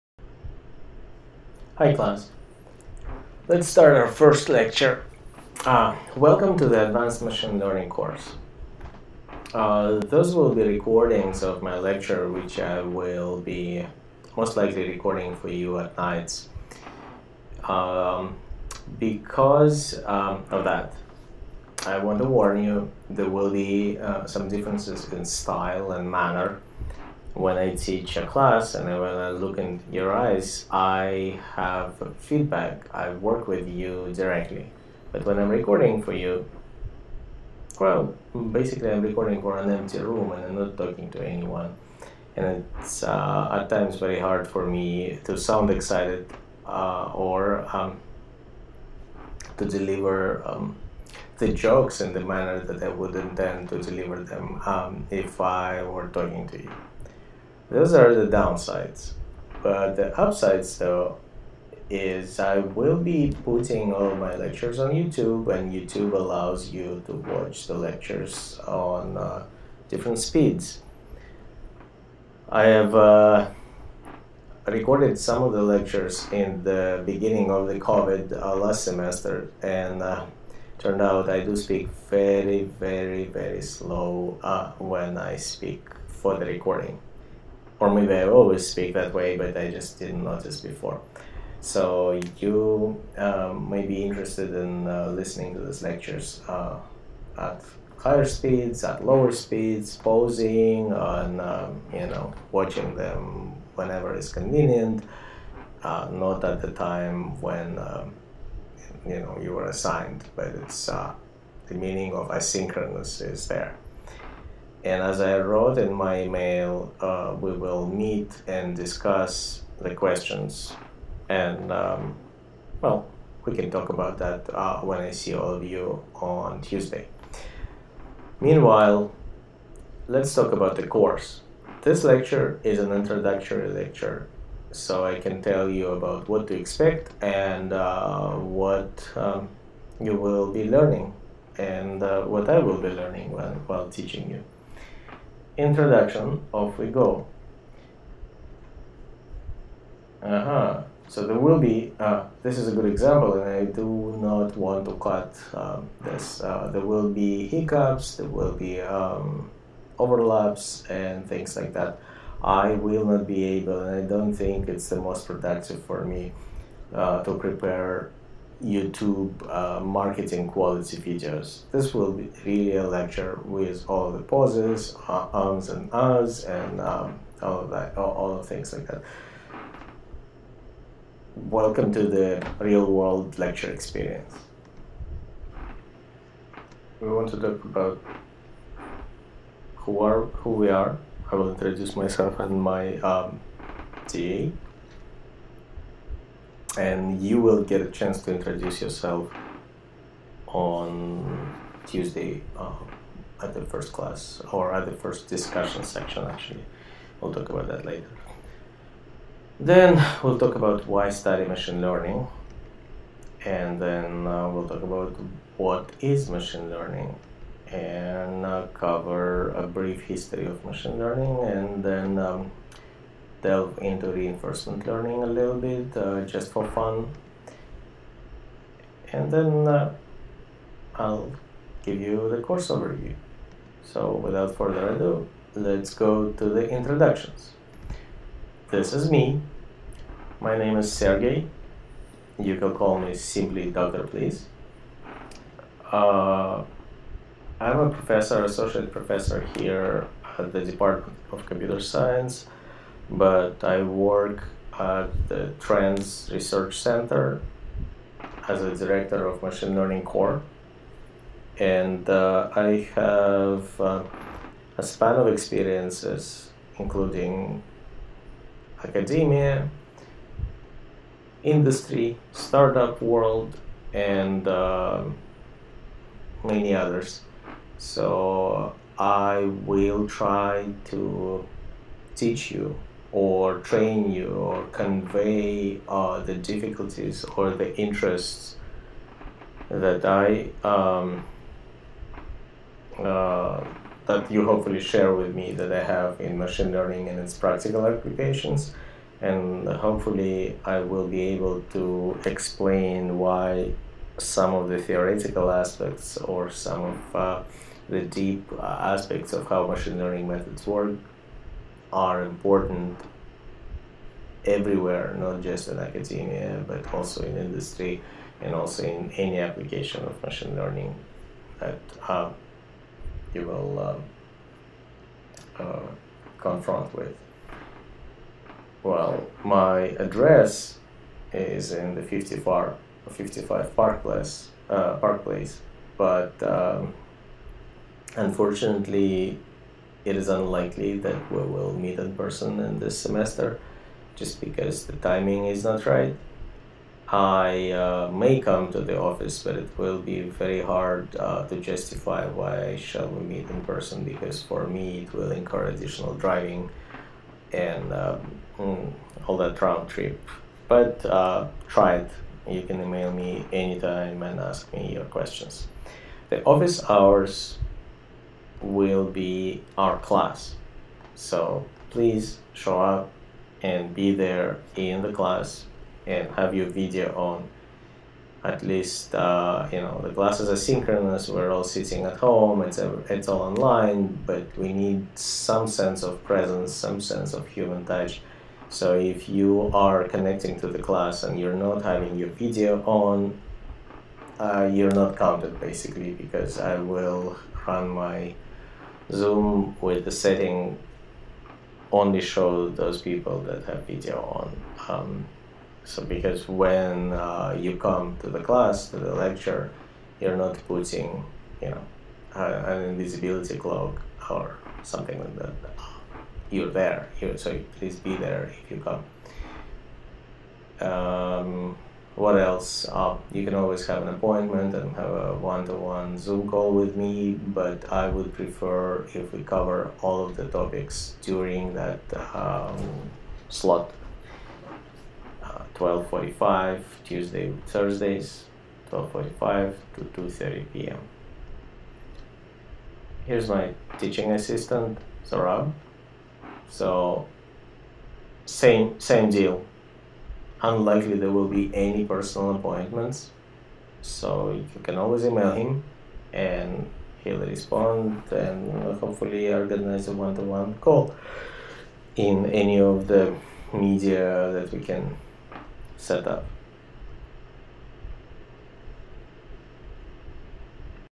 Advanced Machine Learning: Introduction
This is a collection of video recordings I have created for a class I teach.

A short welcome to the class.